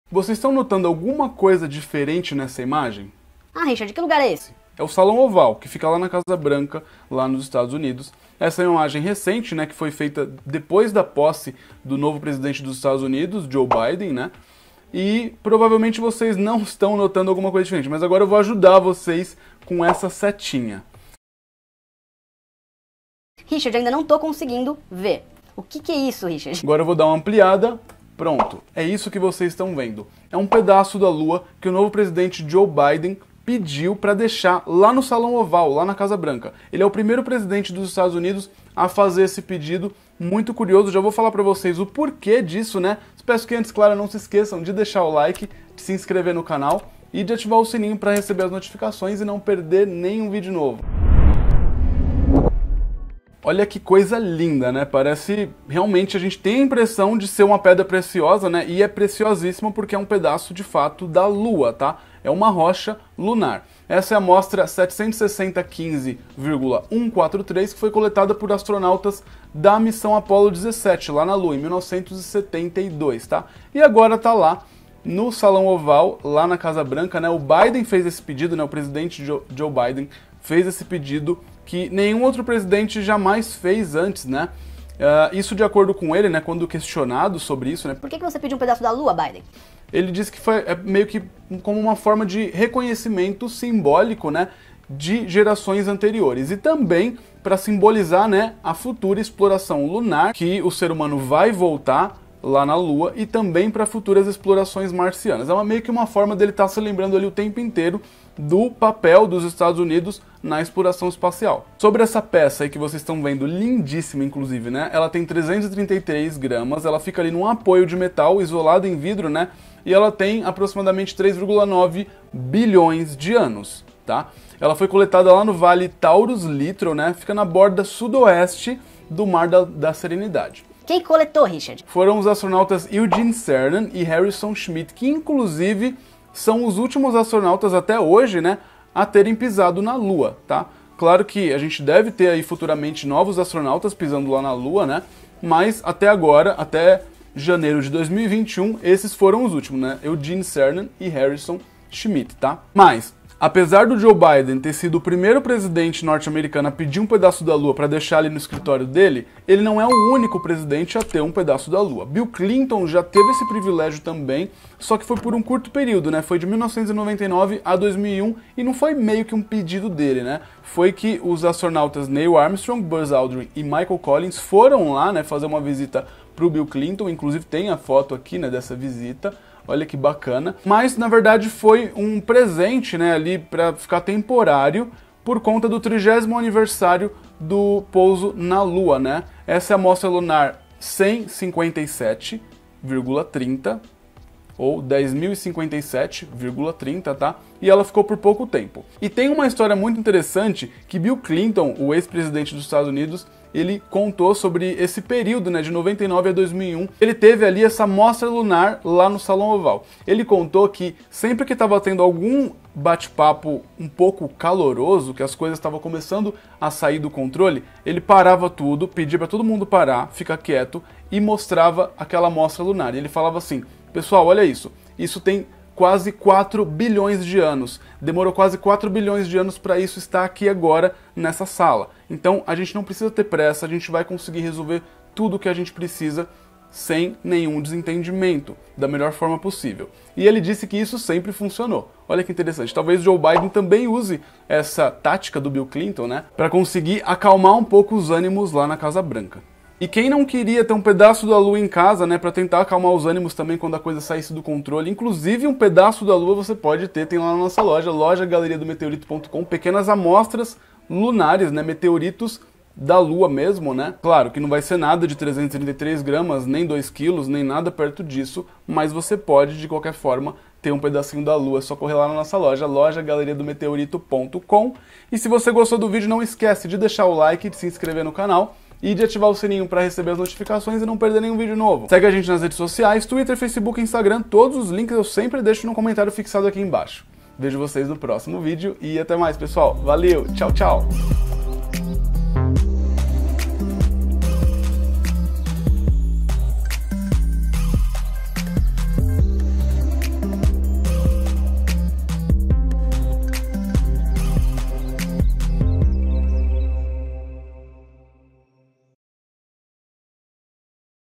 O QUE UM PEDAÇO DA LUA ESTÁ FAZENDO NA CASA BRANCA?!
O presidente dos EUA, Joe Biden, fez um pedido inédito: de uma amostra lunar no Salão Oval da Casa Branca!